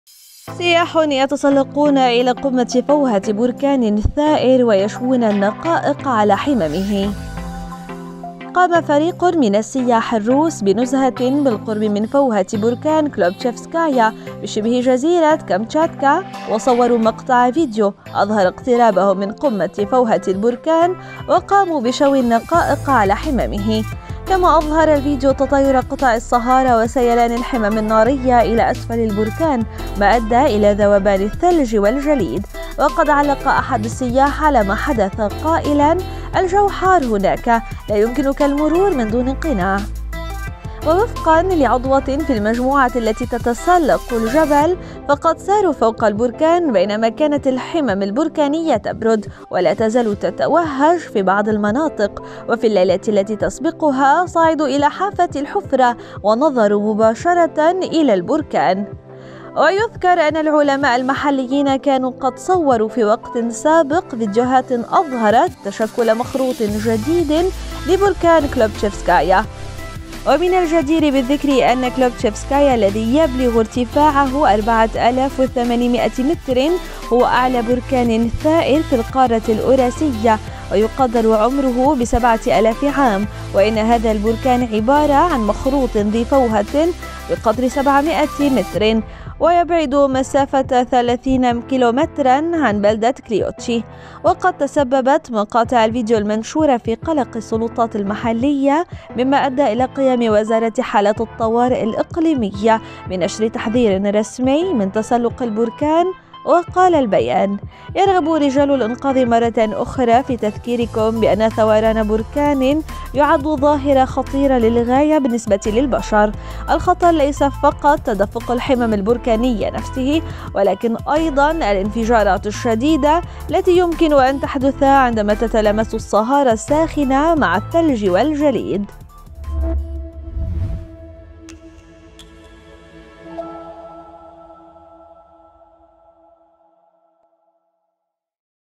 سياح يتسلقون إلى قمة فوهة بركان ثائر ويشوون النقانق على حممه
المصدر: Freedas world
قام فريق من السياح الروس بنزهة بالقرب من فوهة بركان "كلبوتشيفسكايا" بشبه جزيرة كامتشاتكا وصوروا مقطع فيديو أظهر اقترابهم من قمة فوهة البركان وقاموا بشوي النقانق على حممه
كما أظهر الفيديو تطاير قطع الصهارة وسيلان الحمم النارية إلى أسفل البركان، ما أدى إلى ذوبان الثلج والجليد. وقد علق أحد السياح على ما حدث قائلا: "الجو حار هناك، لا يمكنك المرور من دون قناع".
ووفقًا لألكسندرا جونيايفا ، وهي عضوة في المجموعة التي تتسلق الجبل ، فقد ساروا فوق البركان بينما كانت الحمم البركانية تبرد ، ولا تزال تتوهج في بعض المناطق, وفي الليلة التي تسبقها صعدوا إلى حافة الحفرة ونظروا مباشرة إلى البركان
ويذكر أن العلماء المحليين كانوا قد صوروا في وقت سابق فيديوهات أظهرت تشكل مخروط جديد لبركان "كليوتشيفسكايا".
ومن الجدير بالذكر أن "كليوتشيفسكايا" الذي يبلغ ارتفاعه 4800 متر هو أعلى بركان  ثائر في القارة الأوراسية ويقدر عمره بسبعة آلاف عام
وإن هذا البركان عبارة عن مخروط ذي فوهة بقطر 700 متر. ويبعد مسافة 30 كيلومترا عن بلدة كليوتشي
وقد تسببت مقاطع الفيديو المنشورة في قلق السلطات المحلية ، مما أدى إلى قيام وزارة حالات الطوارئ الإقليمية بنشر تحذير رسمي من تسلق البركان
وقال البيان: "يرغب رجال الإنقاذ مرة أخرى في تذكيركم بأن ثوران بركان يعد ظاهرة خطيرة للغاية بالنسبة للبشر". "الخطر ليس فقط تدفق الحمم البركانية نفسه ولكن أيضًا الانفجارات الشديدة التي يمكن أن تحدث عندما تتلامس الصهارة الساخنة مع الثلج والجليد."